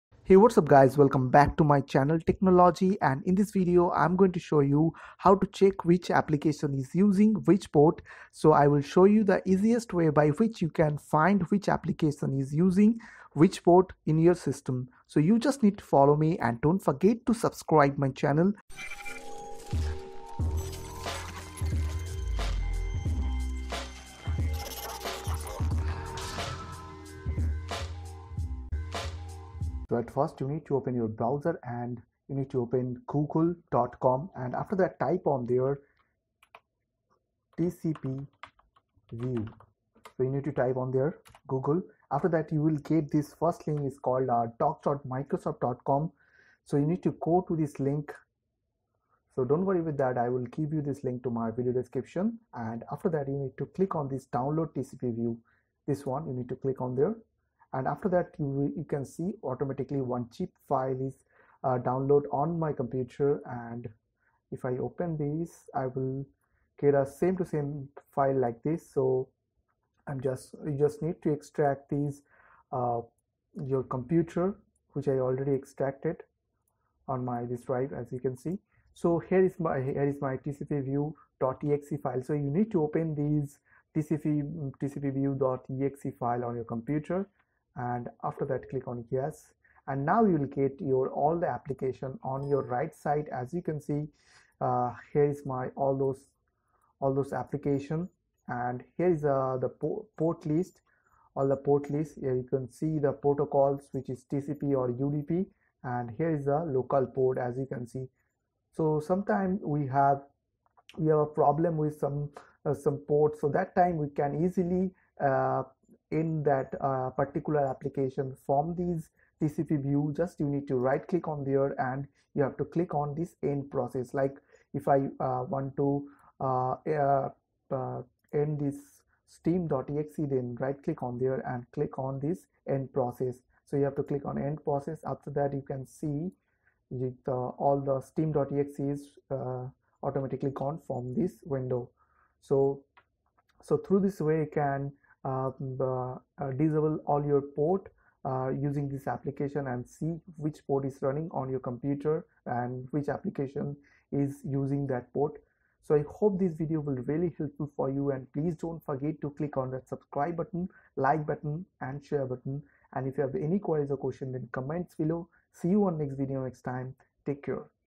How to check which application is using which port
In this video i will show you how to check which port are running in your computer also you can check which application is using which port.